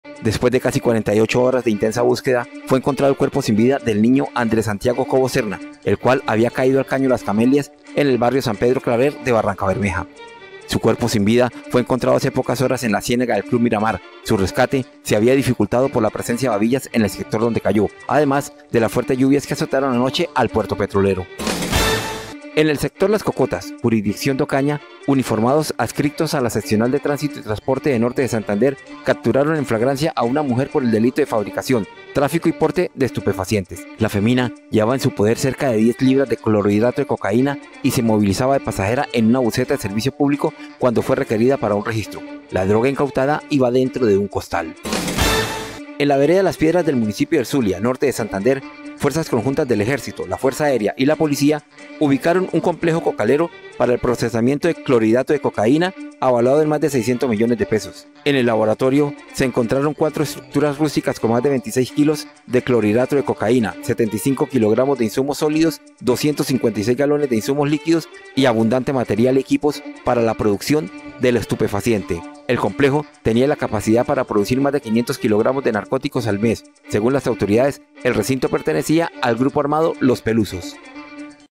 Hallan el cuerpo del menor que cayó a un caño en Barrancabermeja | Oro Noticias
Autoridades y organismos de rescate logran hallar el cuerpo de un niño que había caído al caño Las Camelias, Barrancabermeja. Además, miembros de la Policía Nacional capturaron a una mujer que transportaba 10 libras de cocaína en un costal. Conozca más detalles aquí.